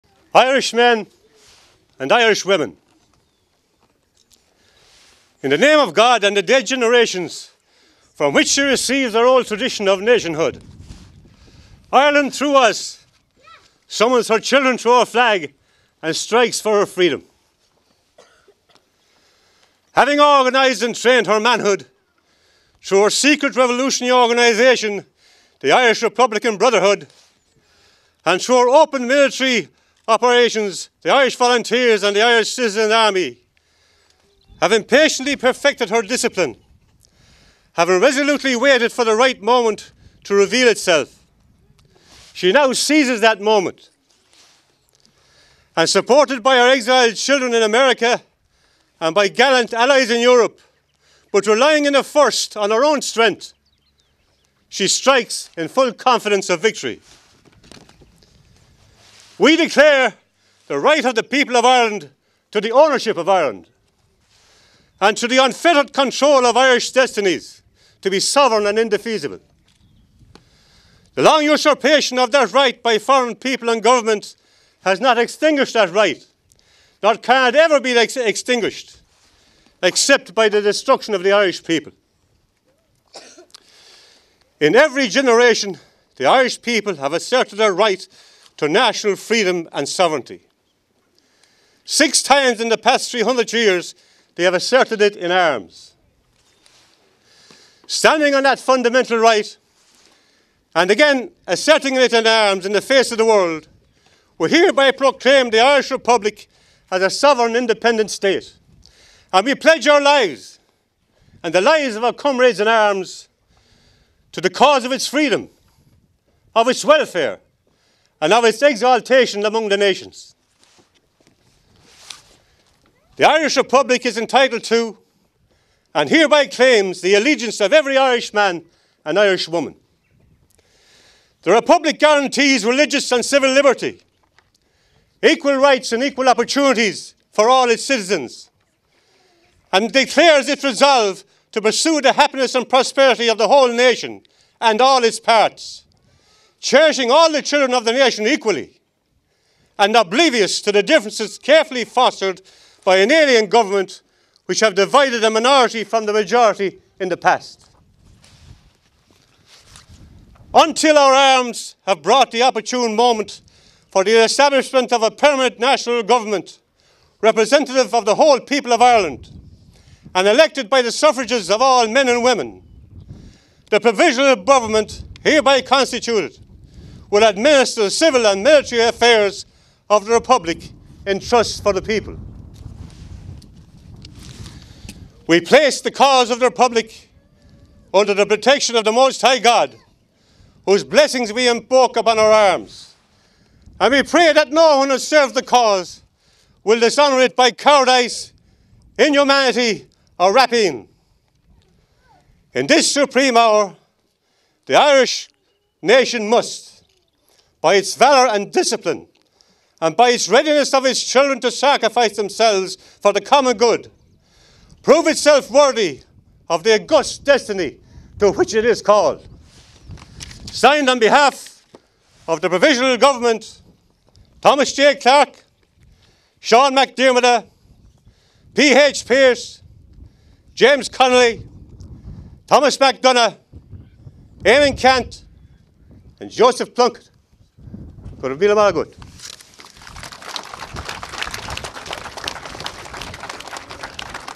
Sean Ryan reads the 1916 Proclamation
1916 Moyglass Commemoration Event.
In this Video Extract - Sean Ryan reads the 1916 Proclamation